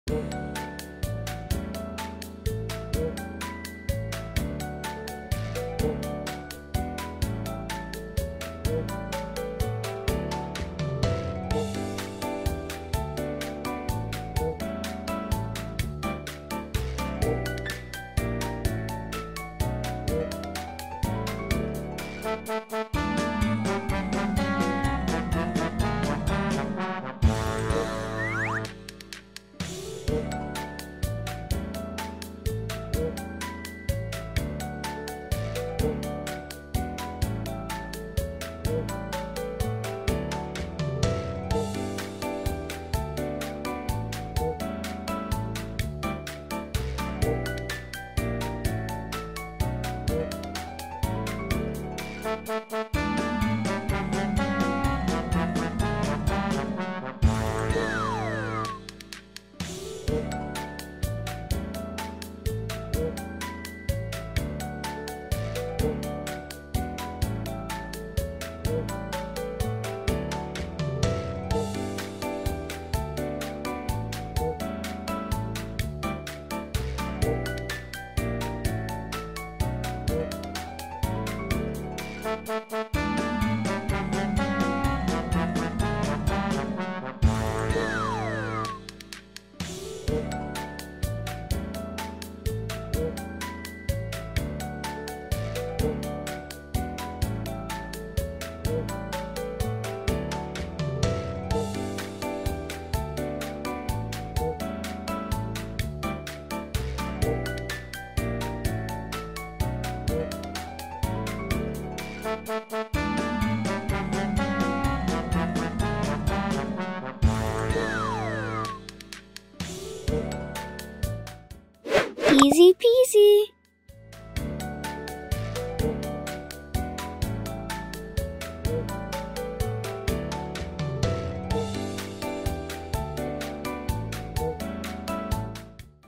HOW TO DRAW FAMOUS YOUTUBER PETS, LAURIDIY MOSSE, EZPZ ideas
From our series "Famous YouTuber pets "
Watch and learn how to draw Laurdiy's dog "Moose" .

Go check out Lauren's channel!